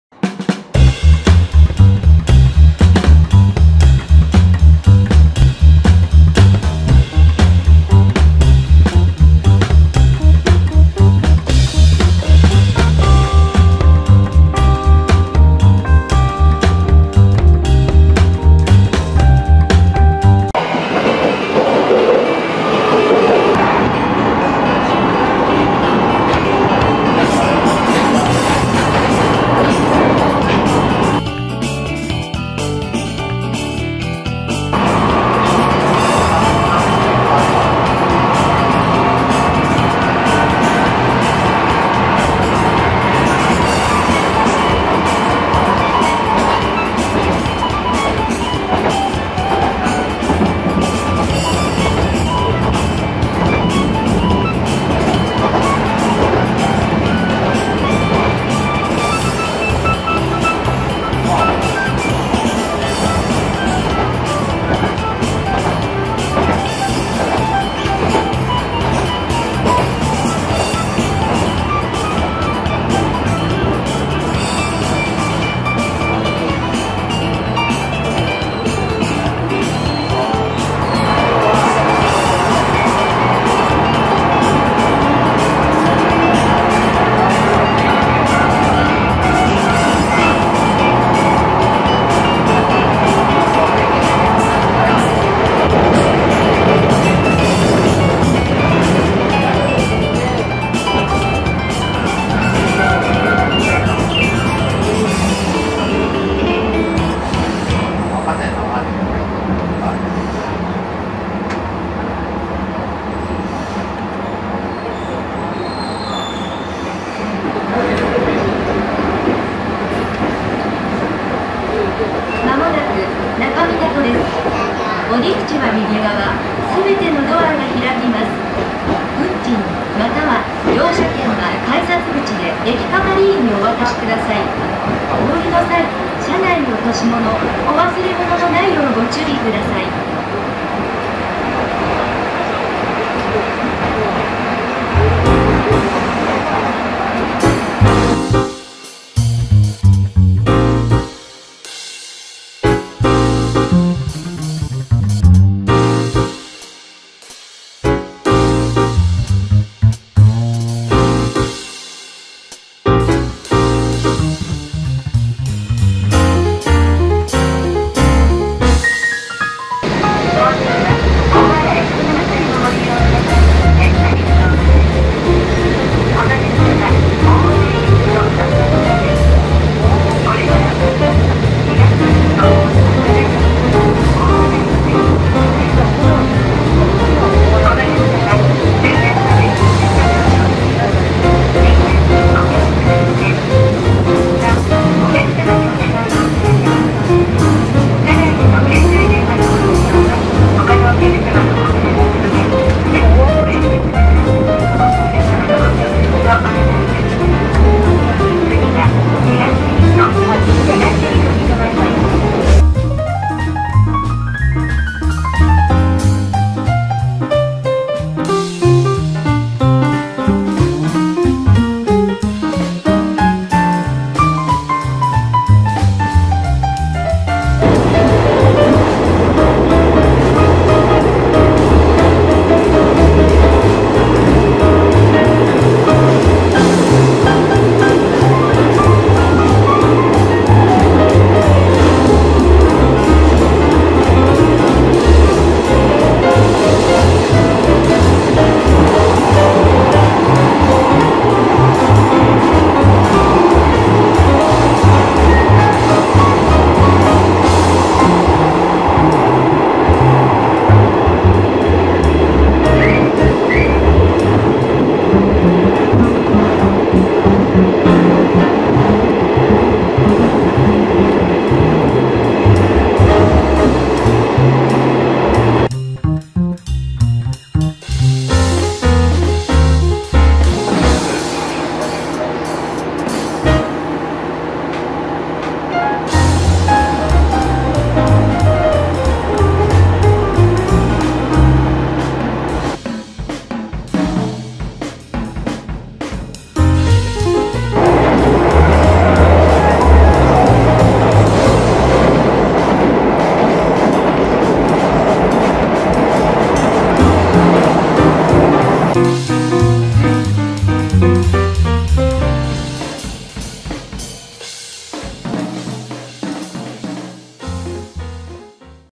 乗り鉄リポート♯２ 茨城ダンガン旅（動画説明欄に使った切符の説明があります)
２０２０年７月24日撮影
肝心なシーンを撮ってなかったので駆け足な動画になってしまいました。。
今回の乗り鉄ではときわ路パスを使用しました。毎年春と秋に発売されいますが今年は新型肺炎感染症の騒動で夏にずれたようです。この切符は、茨城県の鉄道(つくばエクスプレス、筑波観光鉄道は除く)が一日乗り放題で利用できます。夏期間は終わってしまいましたが秋期間は2020年10月３日から12月20日までの土休日に利用できます。詳しい情報はJR東日本水戸支社のホームページをご覧ください。

私のTwitterアカウント→@jyorikurail